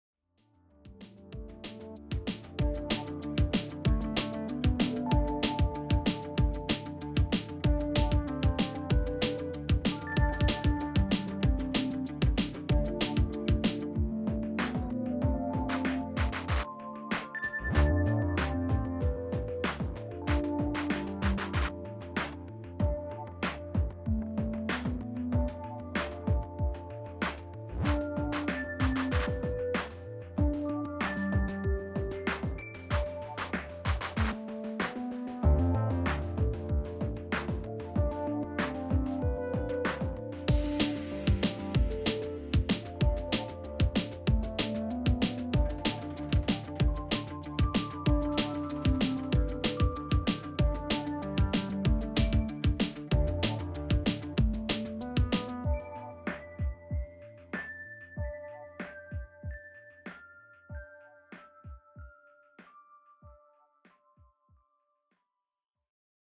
Model Maker Monday: Fanhome Star Trek TNG U.S.S. Enterprise Build Up Kit 74 ages 14+
If you are interested in seeing or purchasing one of the best drones out there I suggest you click this link.